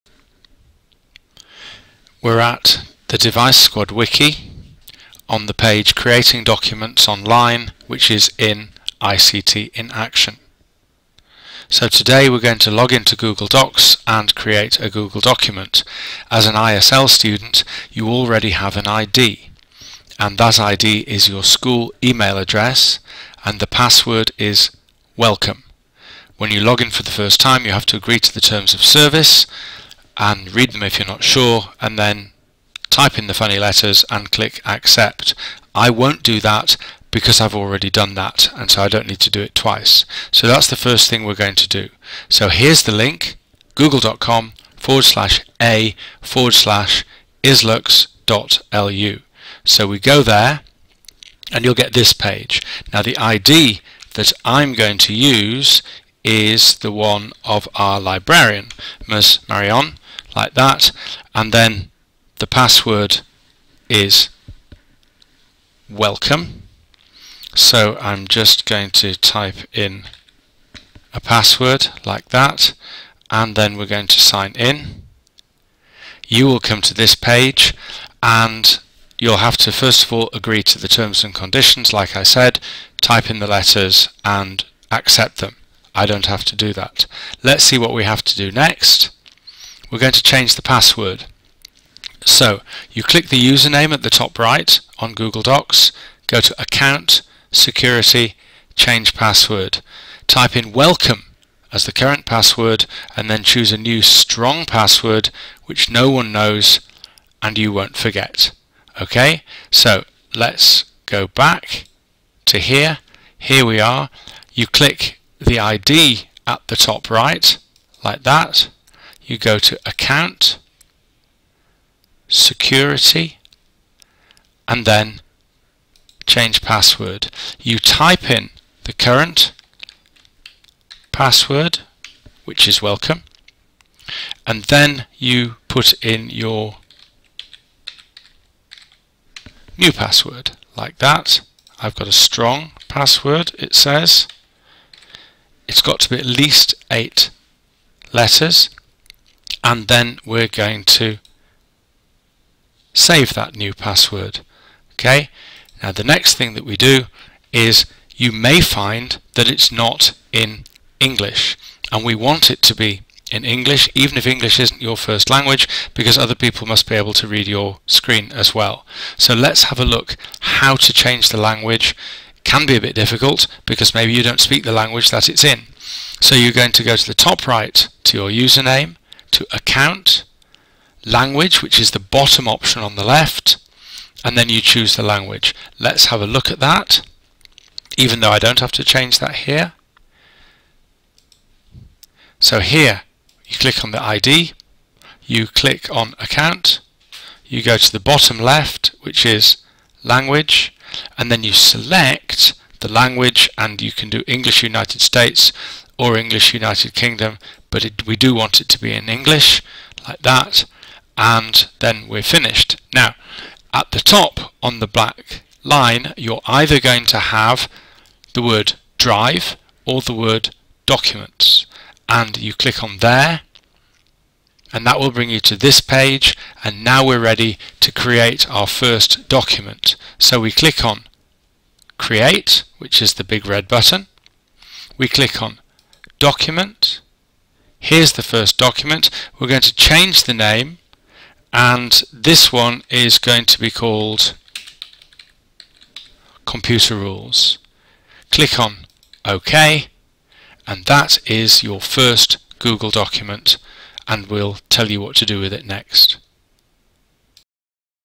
How to create online documents video
How to open your school Google account and create your first document.